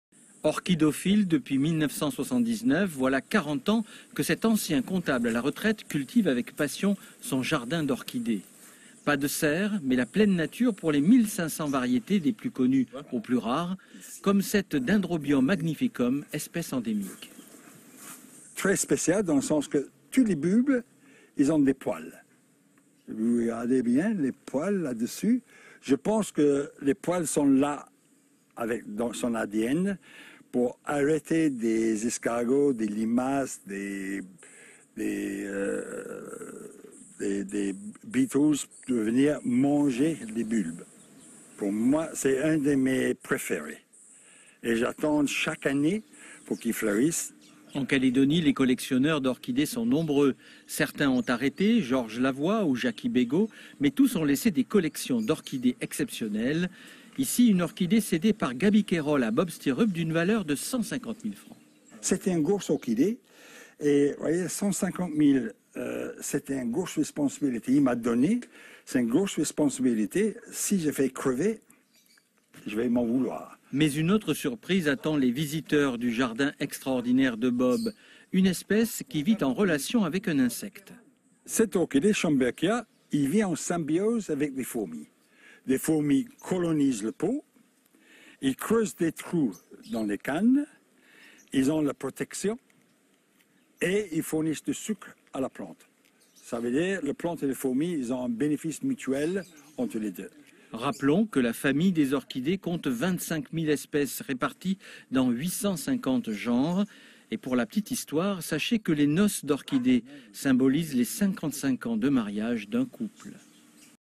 BOB STIRRUP , un orchidophile passionné
Orchidophile depuis 1979 ! Voilà quarante ans que Robert dit Bob Stirrup, ancien comptable, cultive avec passion son jardin d'orchidées. Pas de serres mais la pleine nature mondorienne pour 1500 variétés des plus connues, aux plus rares comme la Dendrobium magnificum, orchidée endémique... à poils. «C'est un de mes préférés et j'attends avec impatience chaque année qu'il fleurisse.»